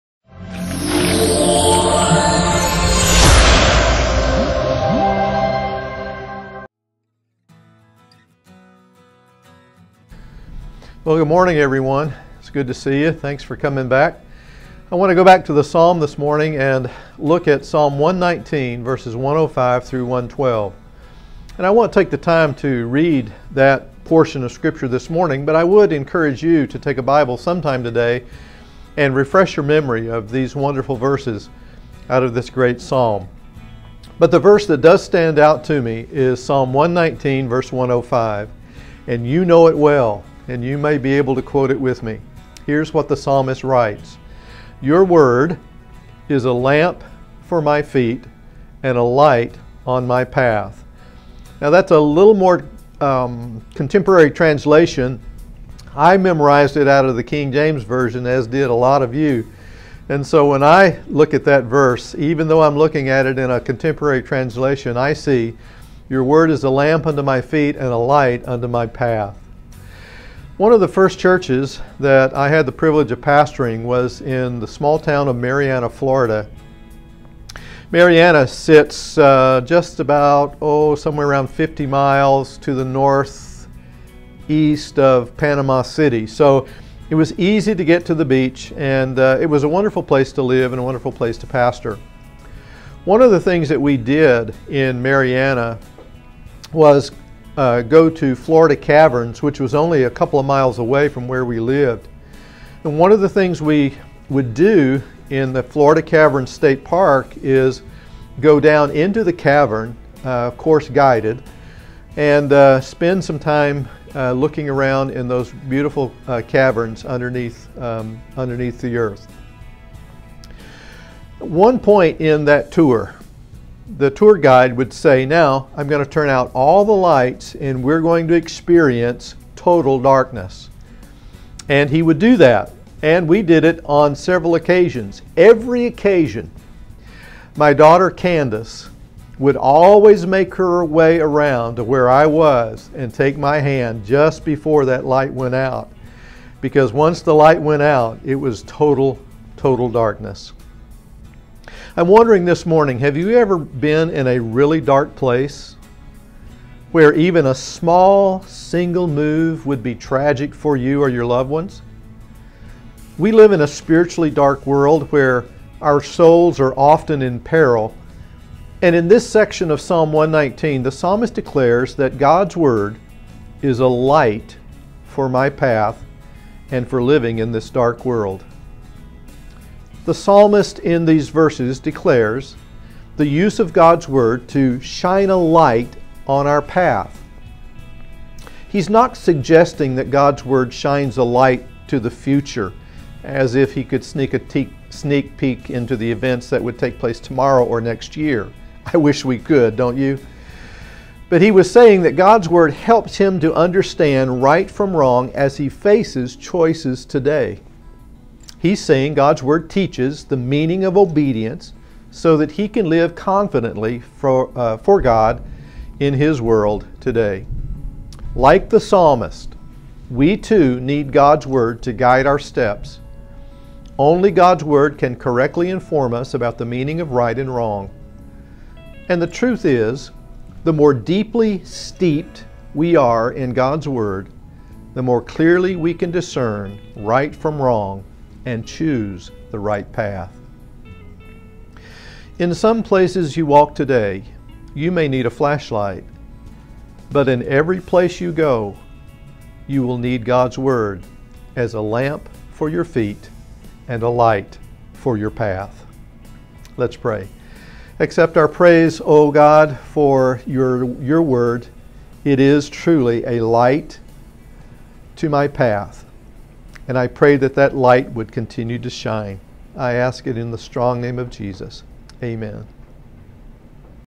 Viral Times 8-20-20
Pastor Don with Psalm 119 "A light unto our Path"